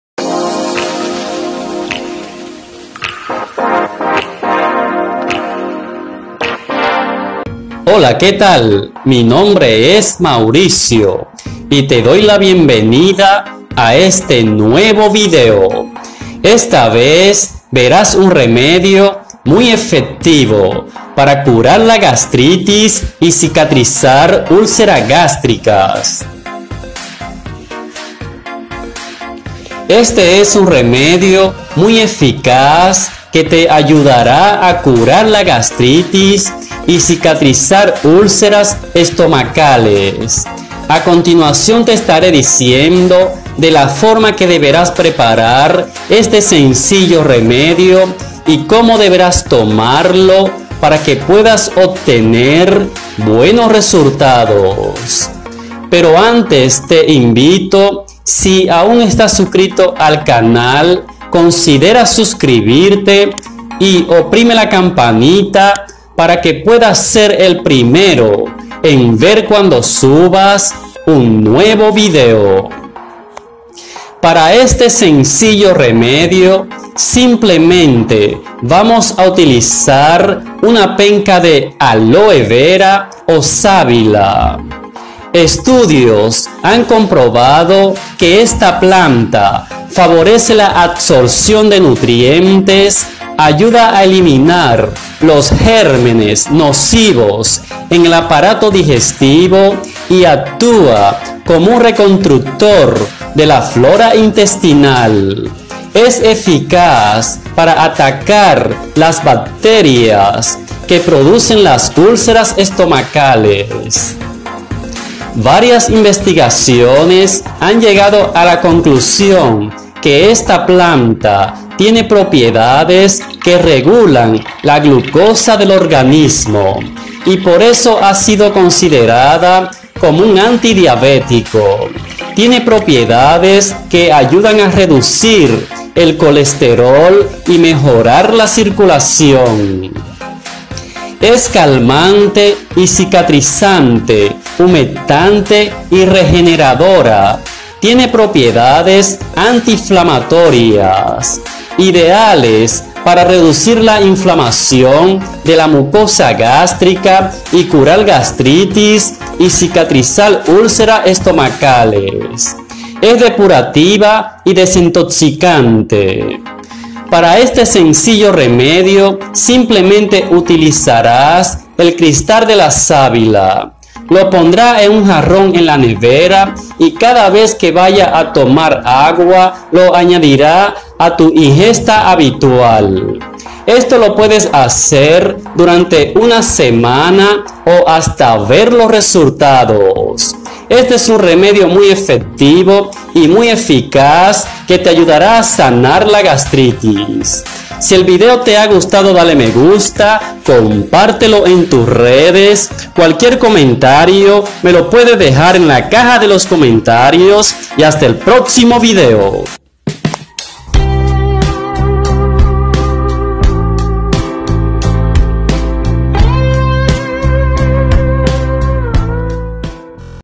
Cura gastritis y cicatriza úlceras gástricas de forma efectiva y natural 2020
Con éste sencillo y excelente remedio podrás Curar gastritis y cicatrizar úlceras gástricas de forma efectiva y natural 2020

Toda la salud de la naturaleza en tus manos.. salud con la naturaleza